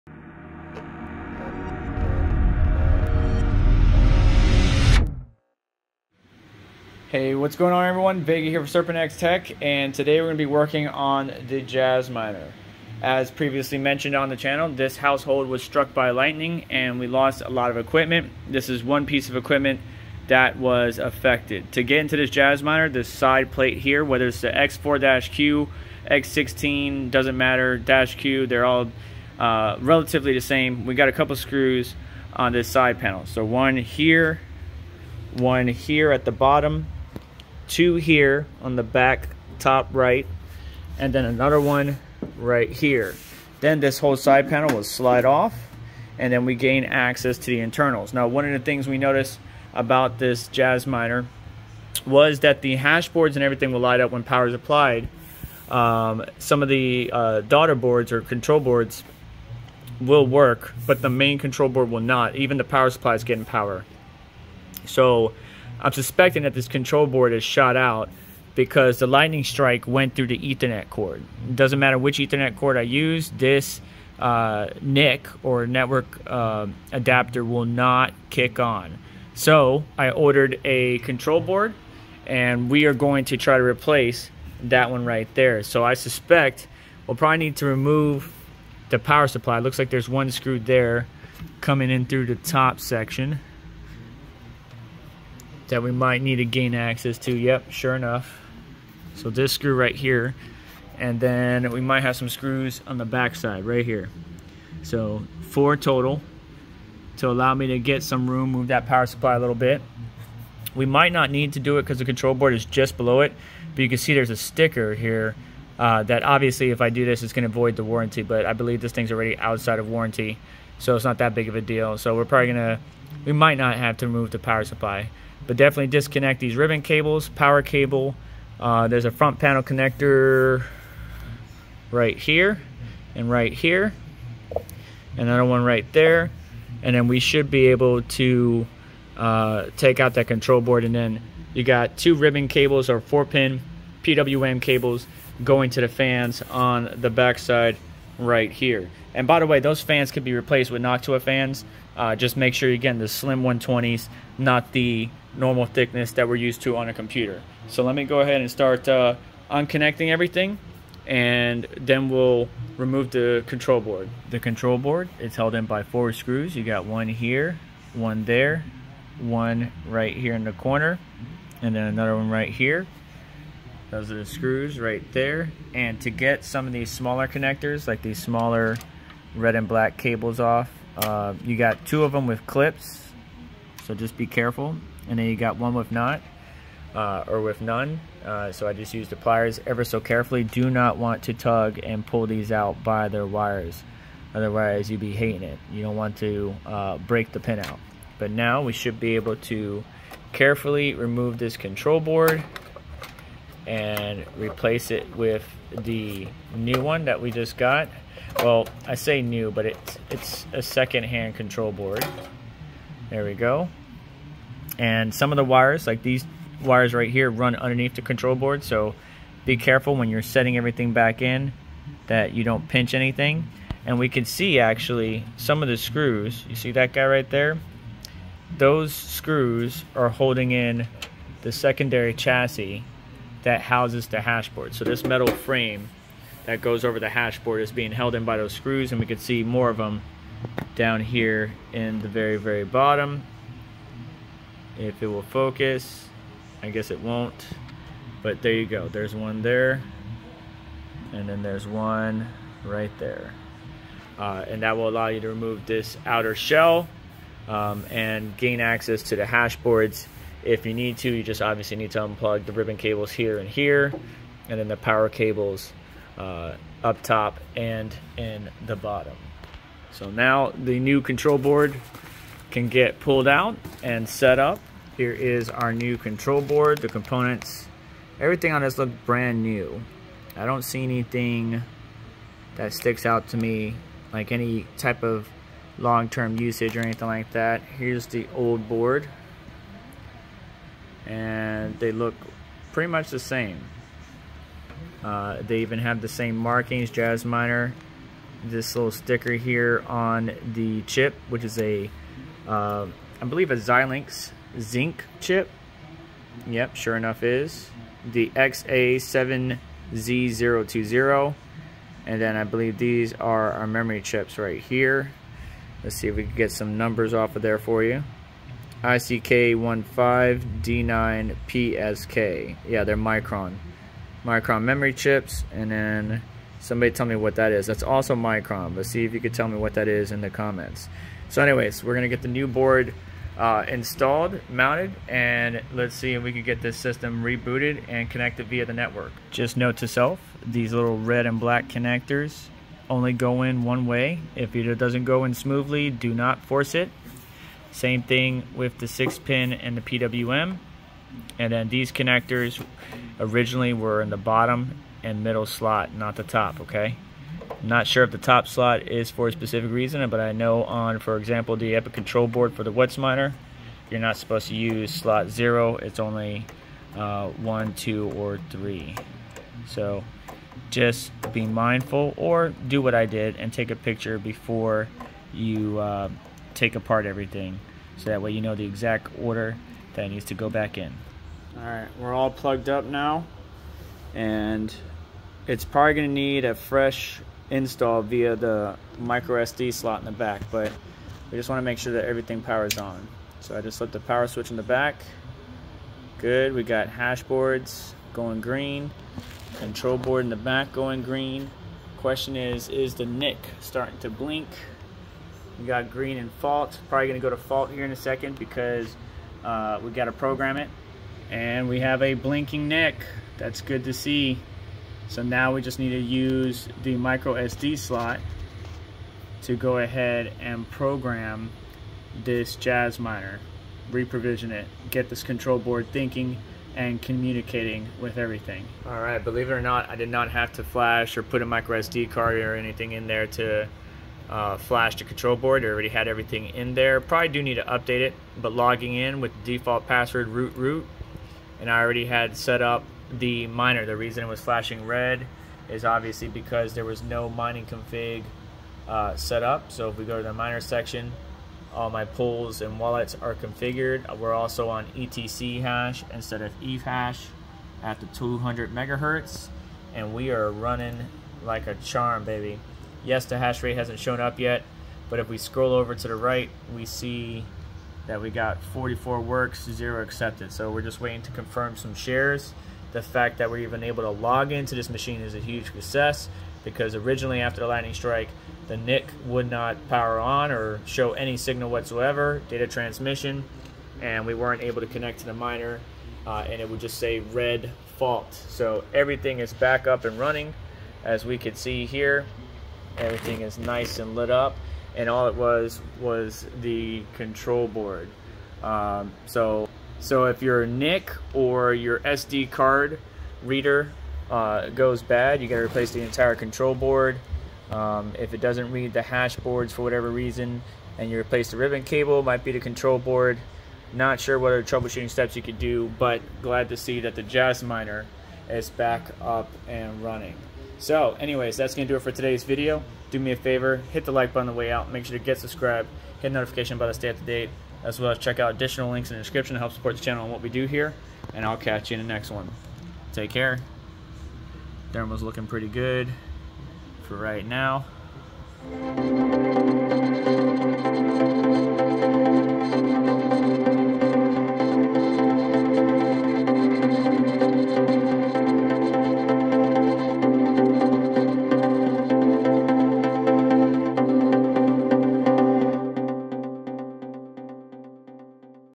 Replacing Control Board on Jasminer X4 Q
Thanks to @JINGLEMINING we go over how to replace the control board in this JasMiner X4-Q. It can mine ETChash and ETHash and is very quiet.

You can buy one of the Latest Jasminers or other options via the